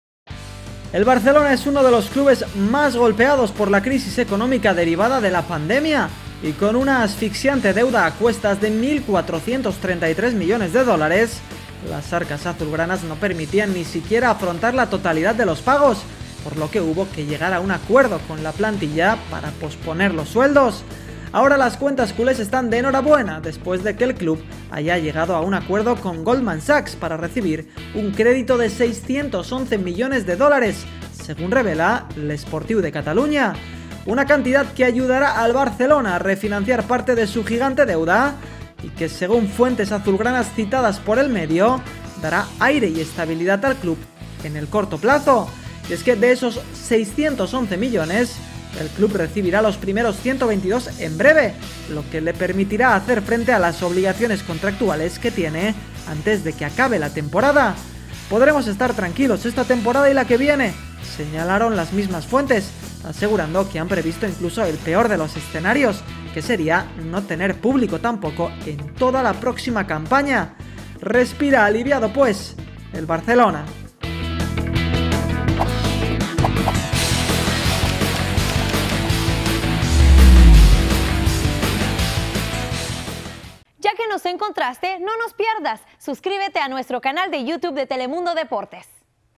El Barça toma aire: crédito de ¡610 millones! | Telemundo Deportes
Video oficial de Telemundo Deportes. El club azulgrana llegó a un acuerdo para recibir un crédito que permitirá a la institución afrontar pagos cruciales a corto plazo.